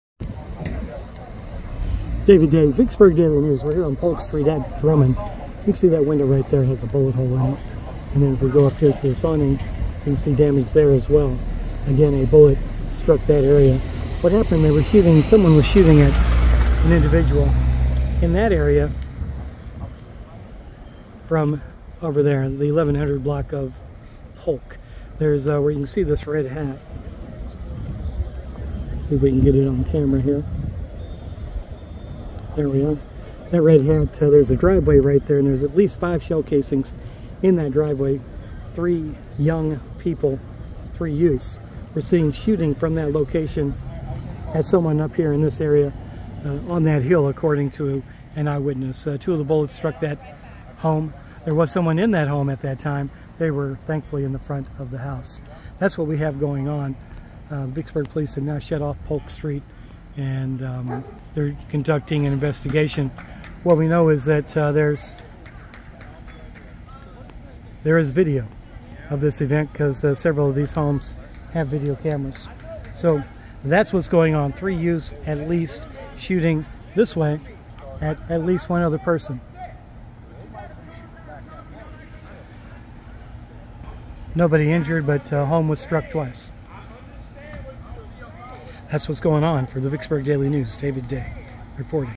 August 9, 2023 Shooting at Polk and Drummond
A shooting at Polk and Drummond has area residents stressed and angry. One resident can be heard yelling at police that nothing is being done to stop the crime.